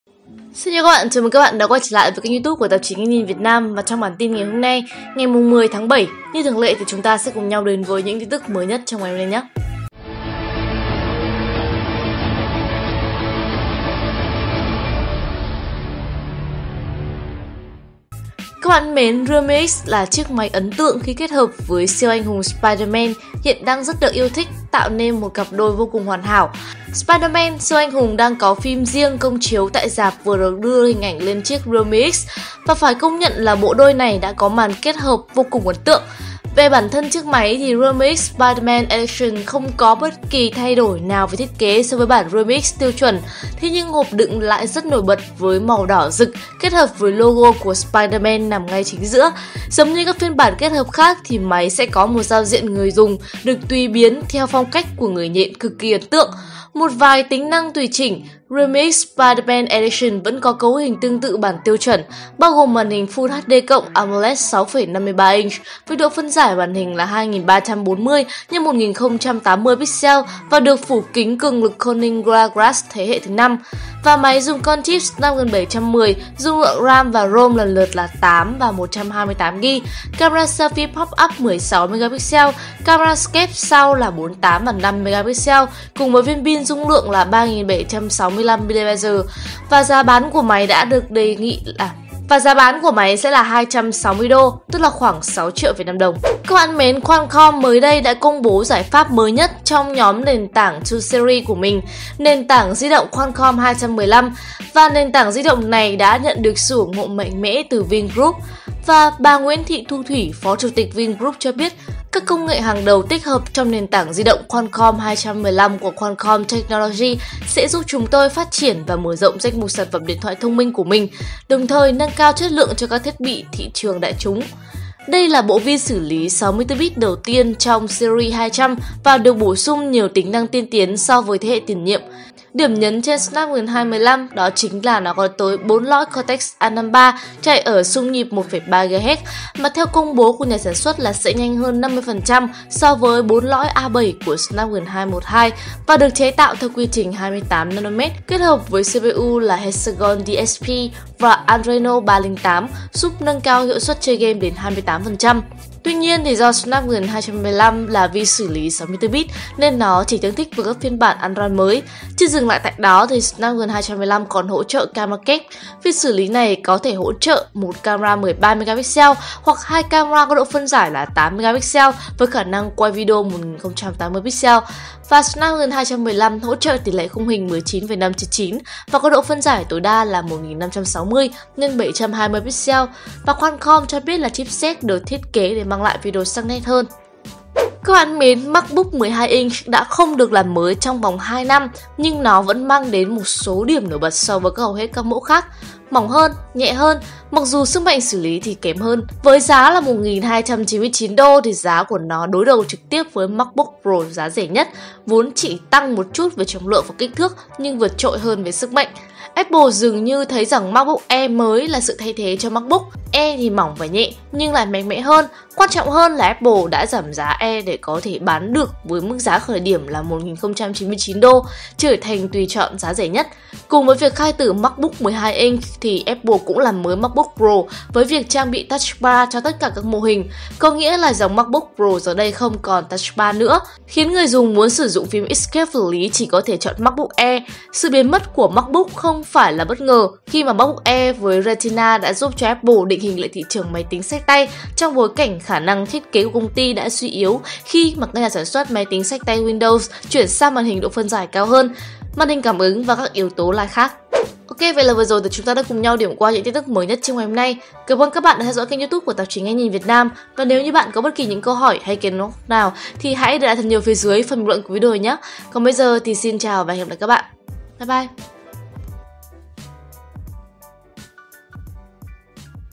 Realme X Spider Man Edition chỉ 260 USD| Vsmart thế hệ mới sẽ trang bị Snapdragon 215 vừa ra mắt
Realme X Spider Man Edition lên kệ giá chỉ 260 USD
Smartphone Vsmart thế hệ mới sẽ trang bị Snapdragon 215 vừa ra mắt
Apple chính thức khai tử MacBook 12 inch chỉ sau 4 năm